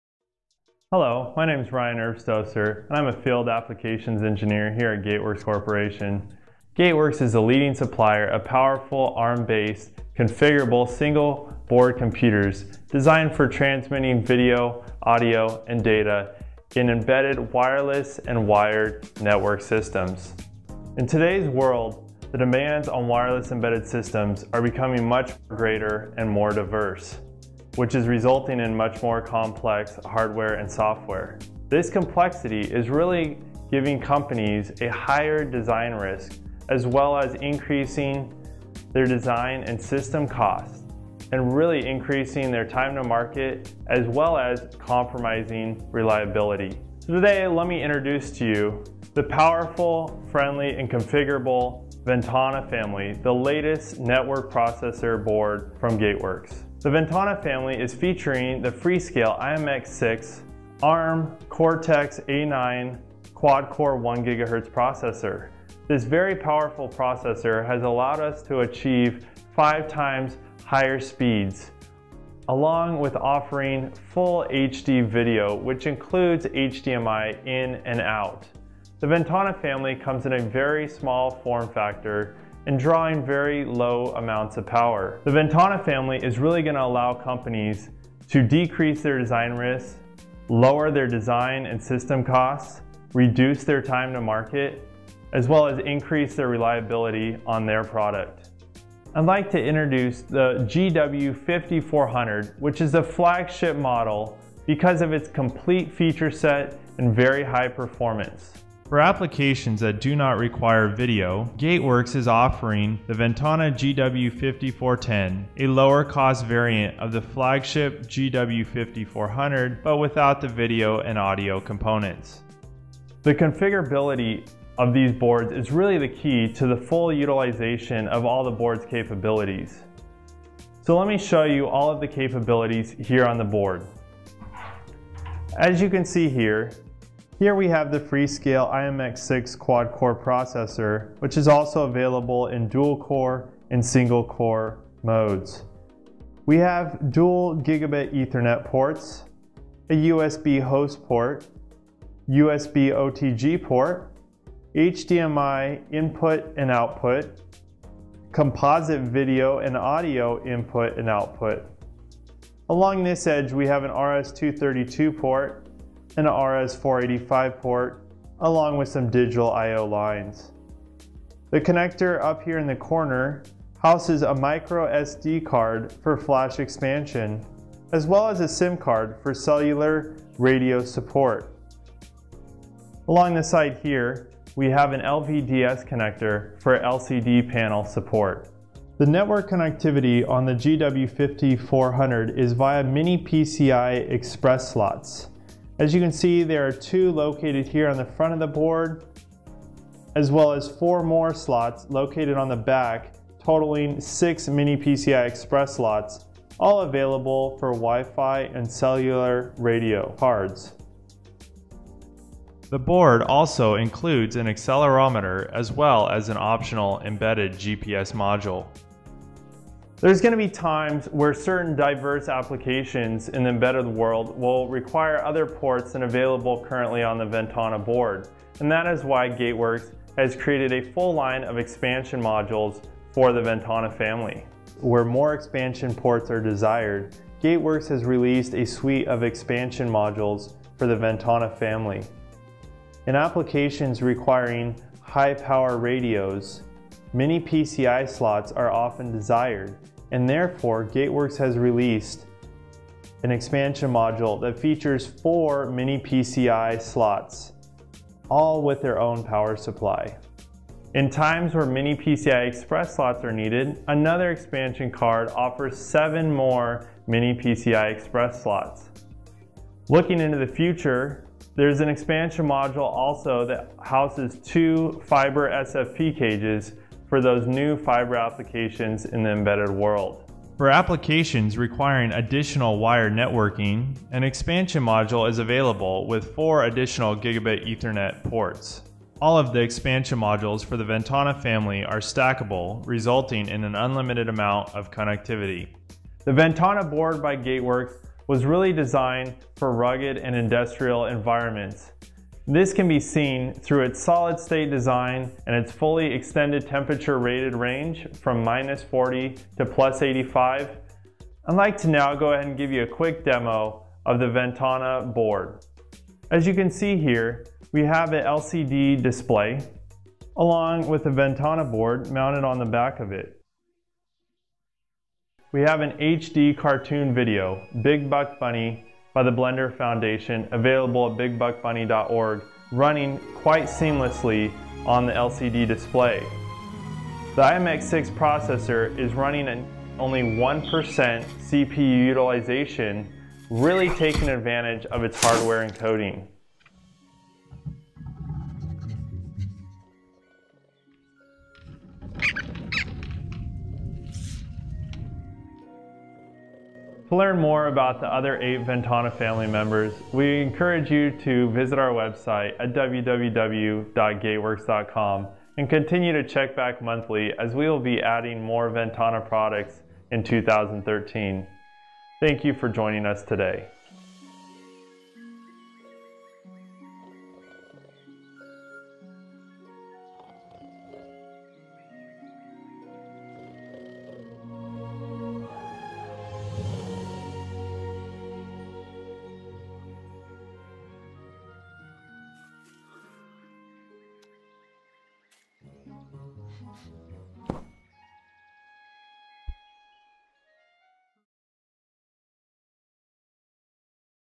Gateworks Ventana GW5400 Embedded Wireless Single Board Computer
Gateworks introduces the new Ventana GW5400, the latest in their network processing boards for wireless embedded systems. Featuring a Freescale IMX6 ARM Cortex-A9 1GHZ Quad core processor, this board has plenty of power for high performance networking, video, and audio applications. The hardware video encoder allows H.264 HD video to be processed flawlessly.